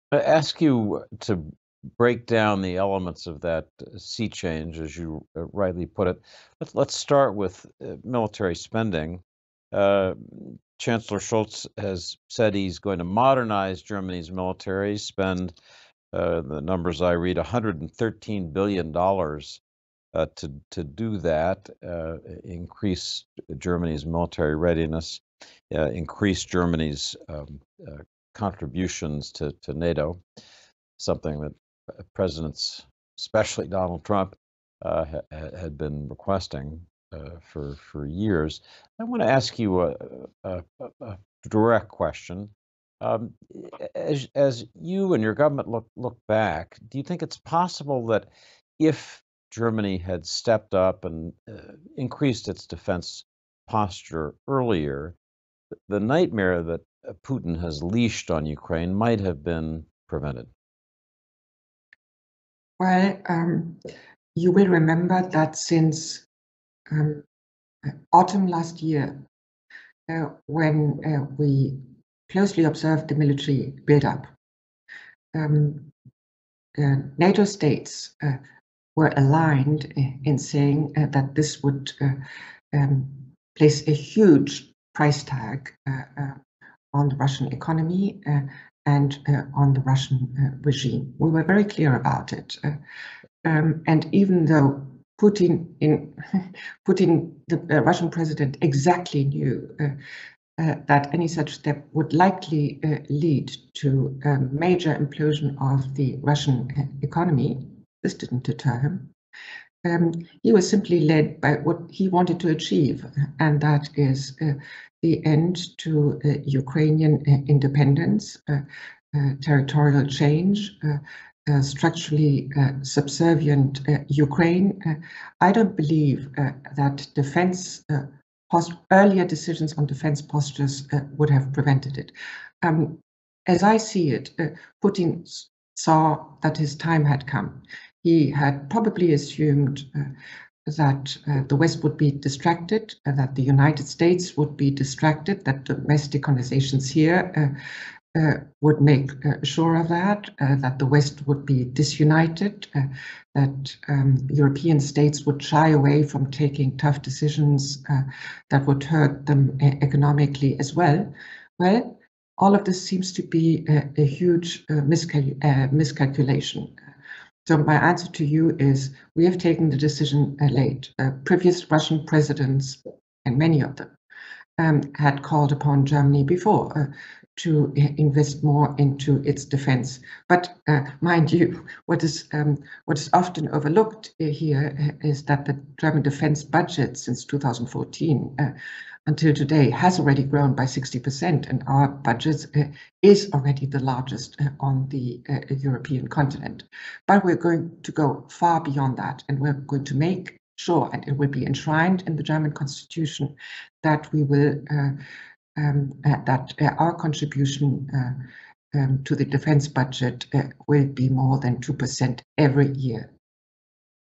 Emily Haber thinks Putin made a ‘huge miscalculation’ in how West would respond to war in Ukraine
“He had probably assumed that the West would be distracted that the United States would be distracted that domestic conversations here would make sure that that the West would be disunited that European states would shy away from taking tough decisions that would hurt them economically as well. But all of this seems to be a huge miscalculation.” – Emily Haber